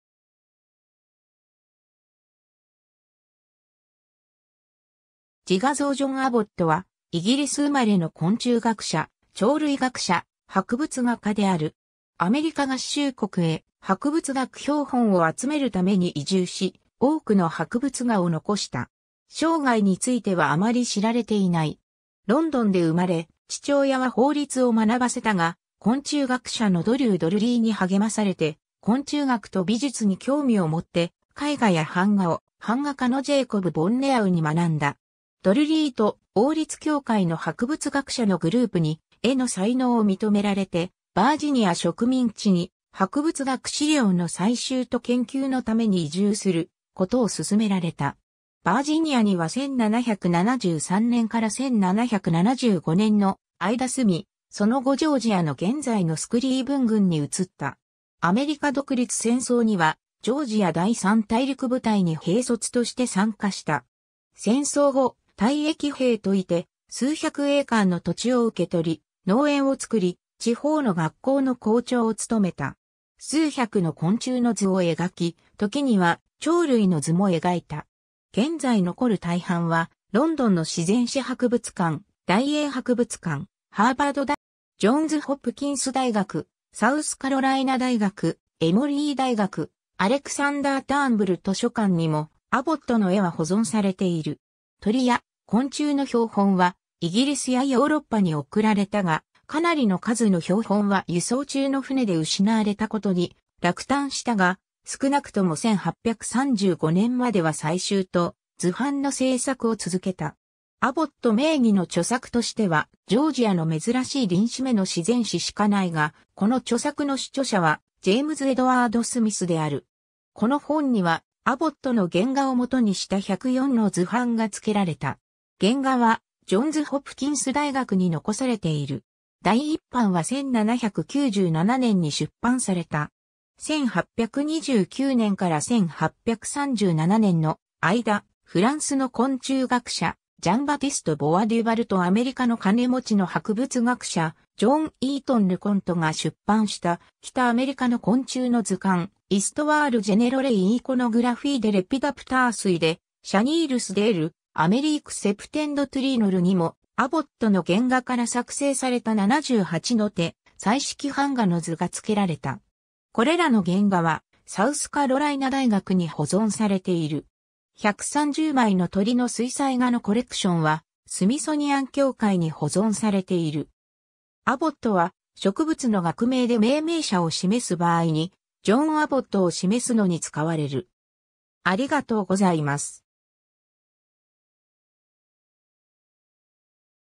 ジョン・アボット (博物学者)
自画像 ジョン・アボット（John Abbot、1751年5月31日 - 1840年12月または1841年1月没）は、イギリス生まれの昆虫学者、鳥類学者、博物画家である。
アメリカ合衆国へ博物学標本を集めるために移住し、多くの博物画を残した。
 生涯についてはあまり知られていない。
ロンドンで生まれ、父親は法律を学ばせたが、昆虫学者のドリュー・ドルリー（Dru Drury）に励まされて、昆虫学と美術に興味をもって、絵画や版画を版画家のJacob Bonneau (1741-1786)に学んだ。
ドルリーと王立協会の博物学者のグループに絵の才能を認められて、バージニア植民地に博物学資料の採集と研究のために移住することを勧められた。
 バージニアには1773年から1775年の間住み、その後ジョージアの現在のスクリーブン郡に移った。
アメリカ独立戦争にはジョージア第3大陸部隊（Third Georgia Continental Battalion）に兵卒として参加した。
戦争後、退役兵といて、数百エーカーの土地を受け取り、農園を作り、地方の学校の校長を務めた。
 数百の昆虫の図を描き、時には鳥類の図も描いた。
現在残る大半はロンドンの自然史博物館、大英博物館、ハーバード大学のホートン・ライブラリに収蔵されている。
ジョンズ・ホプキンス大学、サウスカロライナ大学、エモリー大学、アレクサンダー・ターンブル図書館にもアボットの絵は保存されている。
鳥や昆虫の標本はイギリスやヨーロッパに送られたが、かなりの数の標本は輸送中の船で失われたことに落胆したが、すくなくとも1835年までは採集と図版の製作を続けた。
 アボット名義の著作としては『ジョージアの珍しい鱗翅目の自然史』（"Natural History of the Rarer Lepidopterous Insects of Georgia"）しかないが、この著作の主著者はジェームズ・エドワード・スミスである。
この本にはアボットの原画をもとにした104の図版がつけられた。
原画はジョンズ·ホプキンス大学に残されている。
第1版は1797年に出版された。
 1829年から1837年の間、フランスの昆虫学者、ジャン・バティスト・ボワデュヴァルとアメリカの金持ちの博物学者、ジョン・イートン・ル・コント（John Eatton Le Conte）が出版した、北アメリカの昆虫の図鑑、"Histoire Générale et Iconographie des Lépidoptères et des Chenilles de l’Amérique Septentrionale"にもアボットの原画から作成された78の手彩色版画の図がつけられた。
これらの原画はサウスカロライナ大学に保存されている。
130枚の鳥の水彩画のコレクションは、スミソニアン協会に保存されている。
 Abbotは、植物の学名で命名者を示す場合にジョン・アボット (博物学者)を示すのに使われる。